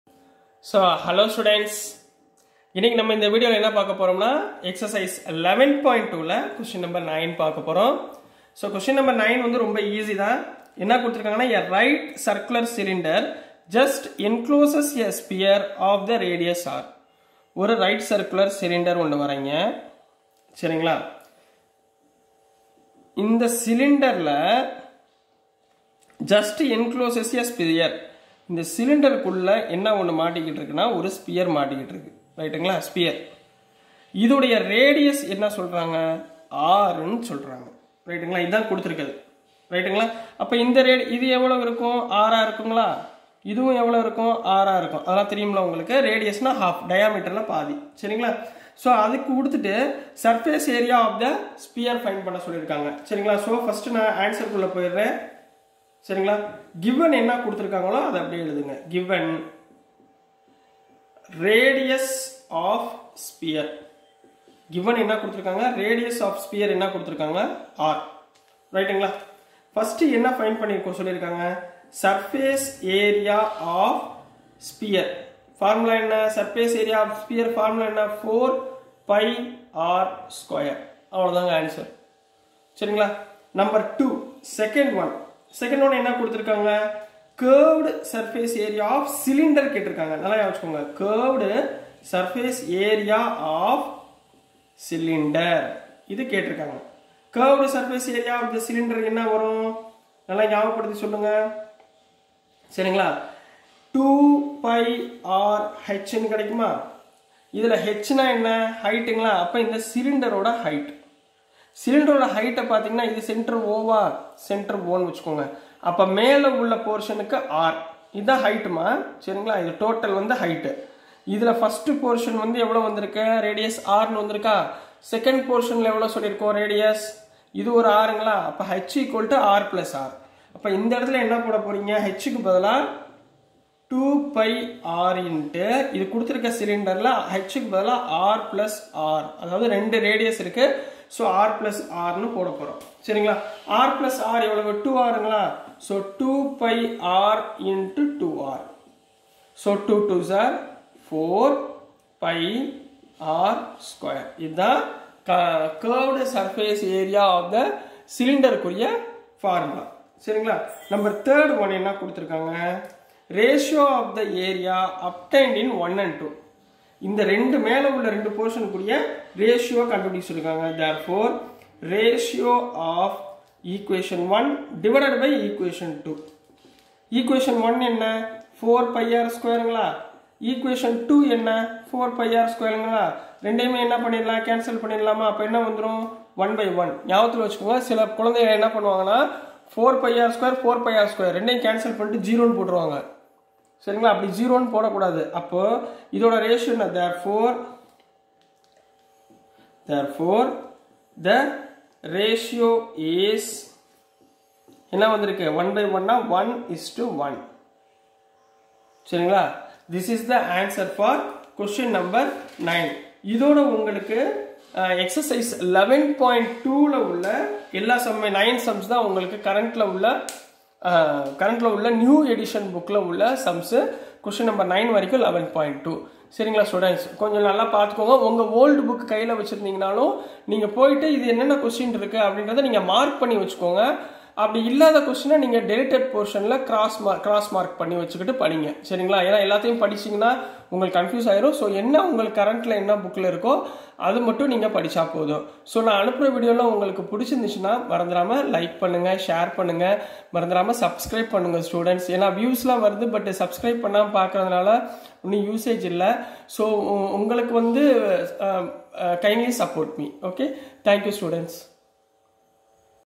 CLASS 9 EX 11.2 Q 9 - MATHS CHAPTER 11 SURFACE AREAS AND VOLUMES - CBSE NCERT NEW SYLLABUS.
Explained about CLASS 9 CBSE NCERT NEW SYLLABUS MATHS CHAPTER 11 SURFACE AREAS AND VOLUMES - EXERCISE PROBLEMS in tamil language.